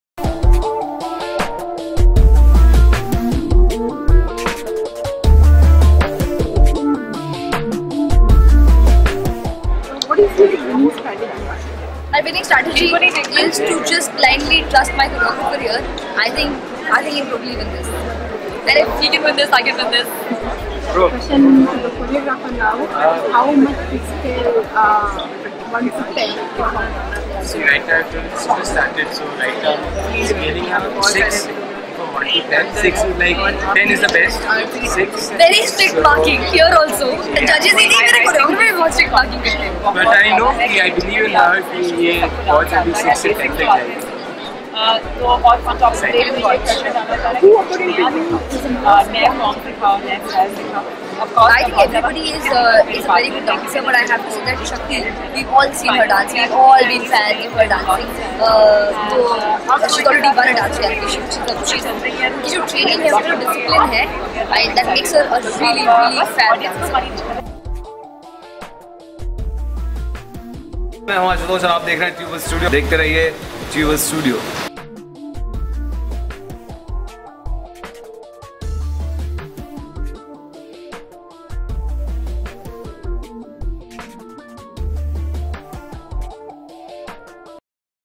Kritika Kamra-Savio Barnes to perform together in Jhalak Dikhla Jaa 7
Kritika Kamra-Savio Barnes to perform together in Jhalak Dikhla Jaa 7.We thought, staying true to the tradition of Savio getting the heaviest partner, he would be assigned to Palak! Savio has choreographed for Bharti Singh, Rapper Hard Kaur, Parul Chauhan, Shweta Tiwari and Ammaji Meghna Malik in former seasons, But This Time Its Kritika Kamra.